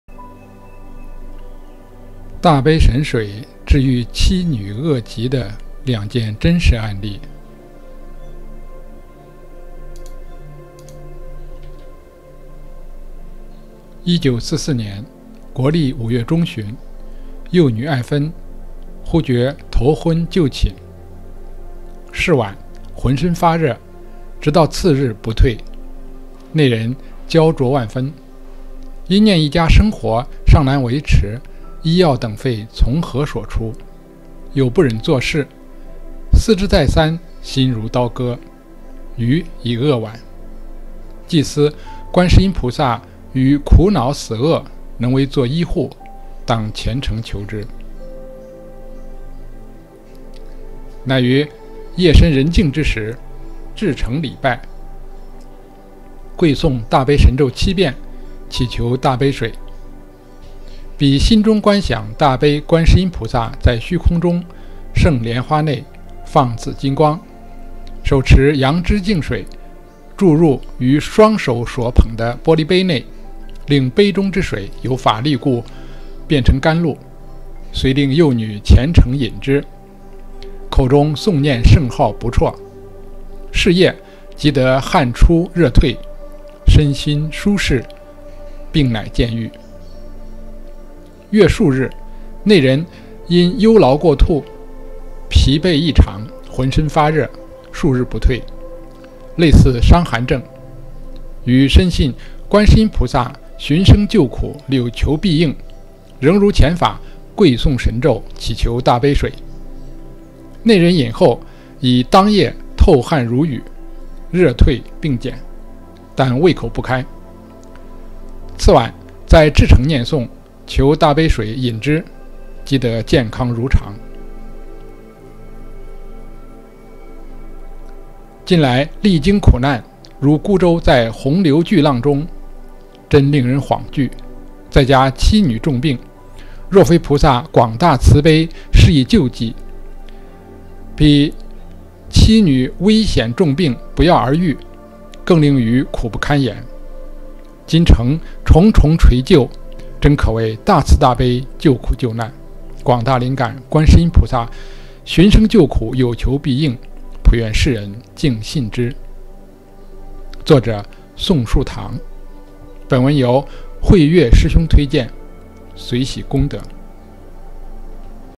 大悲神水治愈妻女恶疾的两件真实案例（宋树棠）
这是1944年民国时代的案例。